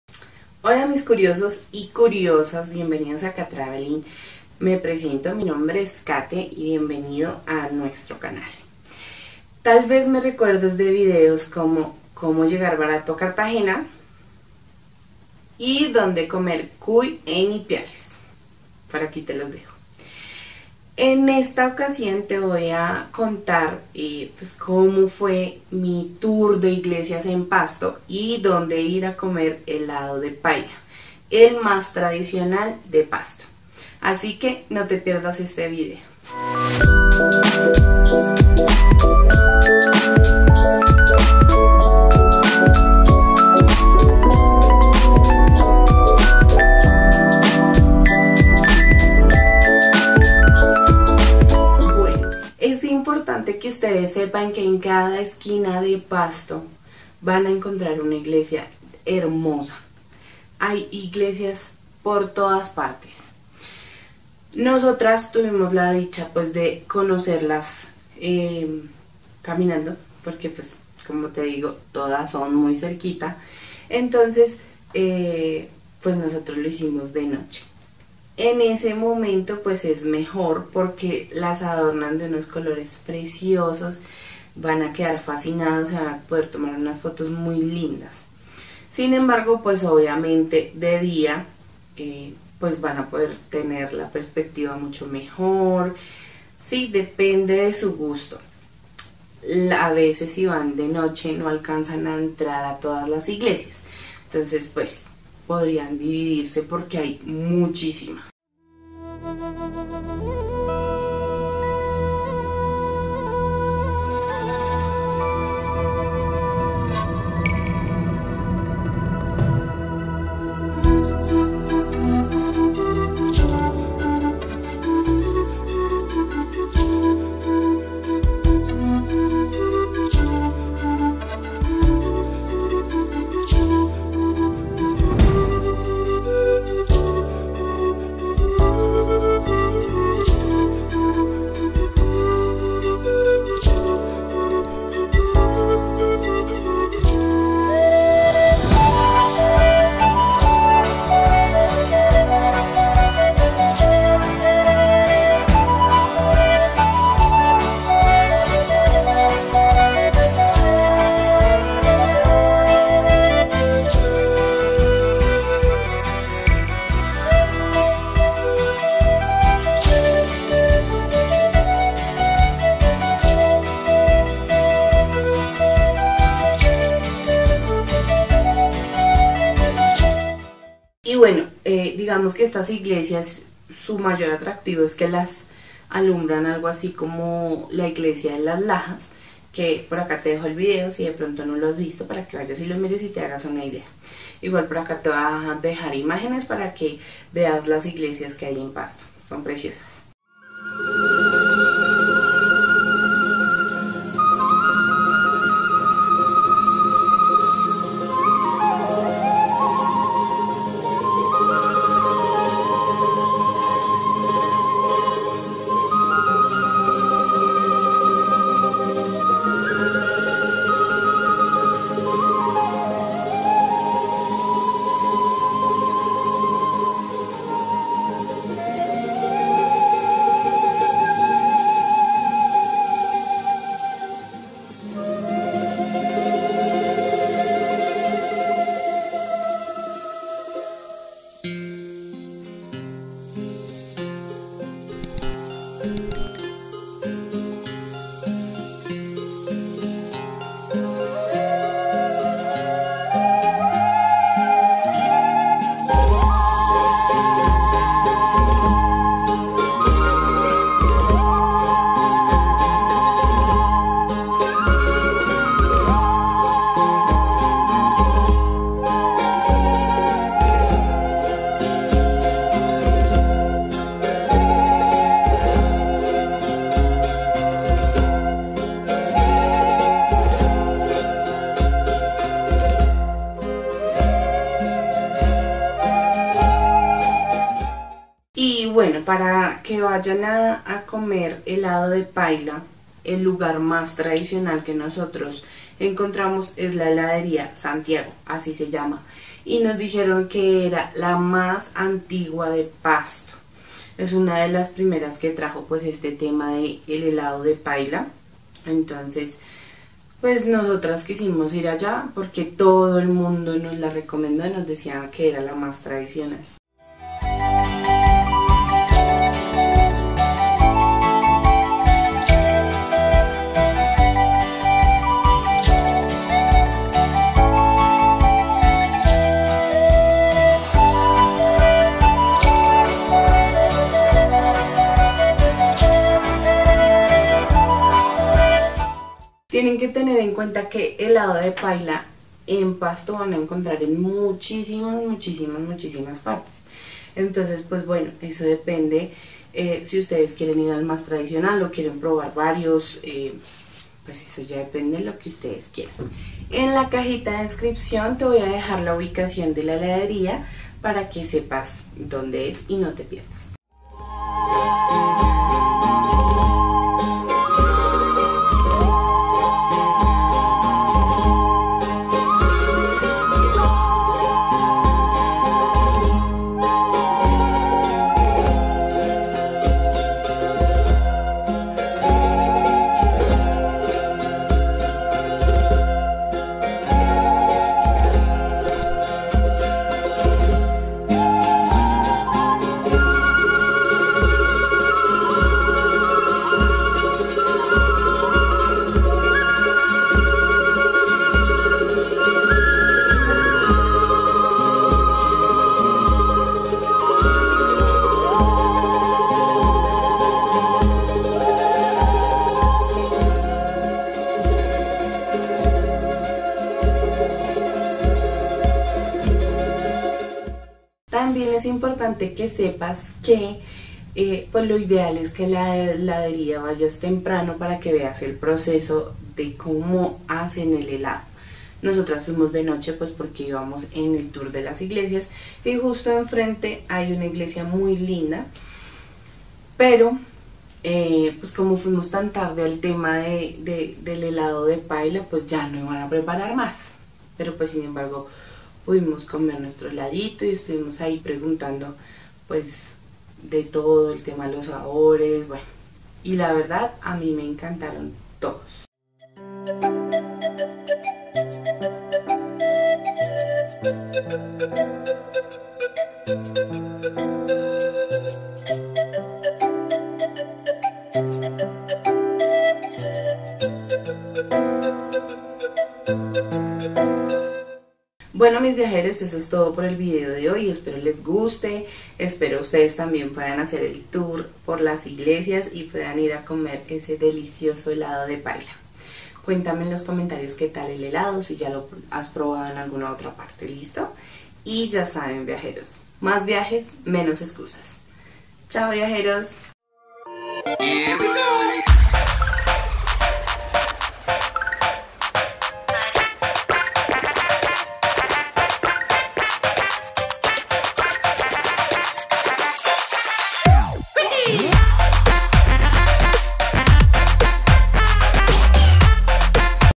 Que hacer en pasto | dónde comer helado de Paila 🍧
Holi mis curiosos del mundo!!!

te quiero contar que viajar barato si es posible
te explico en este video cómo llegar a Baru por tu cuenta en caso de que te quieras quedar alla te cuento dónde es mejor quedarte.

espero te sea de gran utilidad

aquí te dejo los datos de los hospedajes

➕VIAJES
➖EXCUSAS